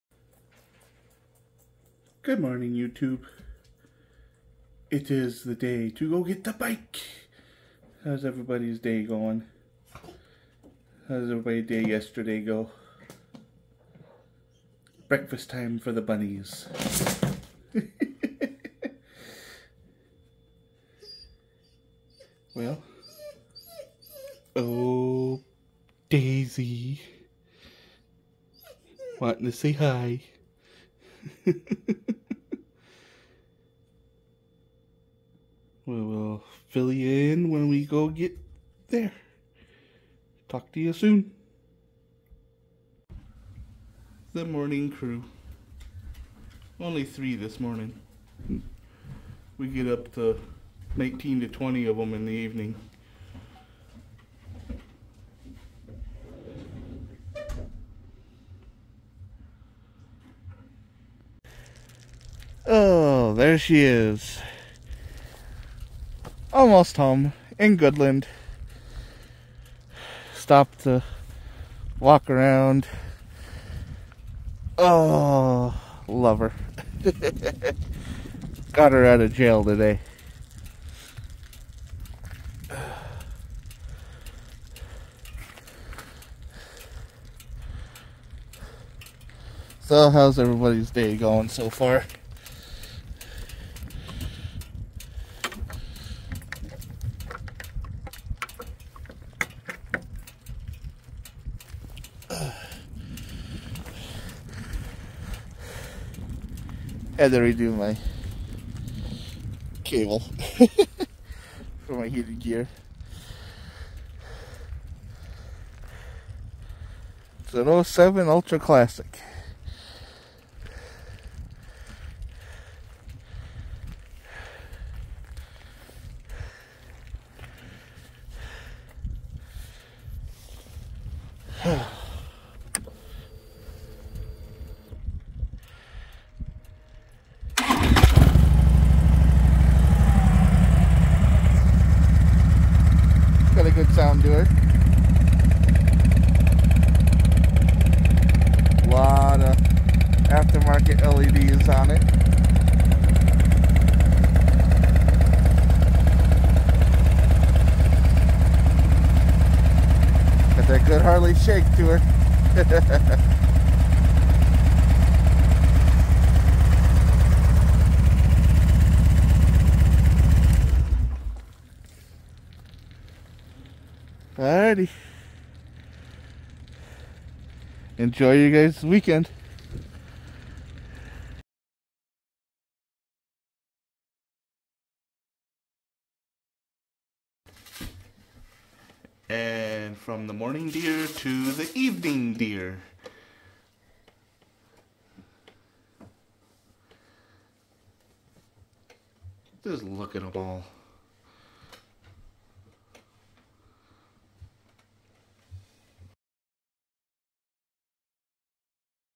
Daily Vlog 04-08-2023 New Bike Day
2007 Harley-Davidson Ultra Classic

shout out to Harley-Davidson Sport Center in Duluth, MN for hooking me up with this bike.